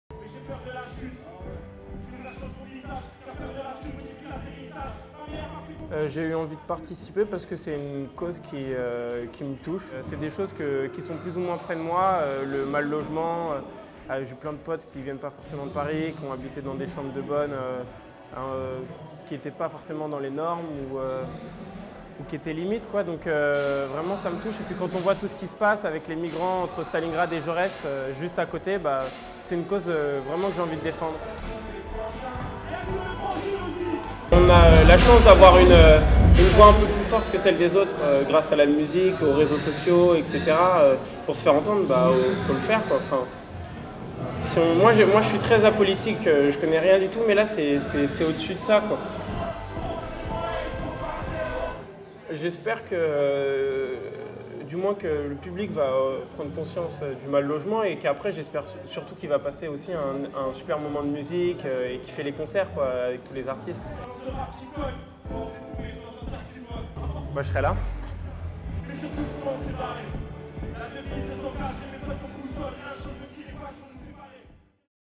GEORGIO, Concert Abbé Road #3 à La Cigale
Fidèle soutien de la Fondation Abbé Pierre, le jeune rappeur a mis une nouvelle fois son talent au service de la lutte contre le mal-logement, à l’occasion de la 3e édition du concert Abbé Road #3 sur la scène de La Cigale. Entre deux balances, il s’en explique.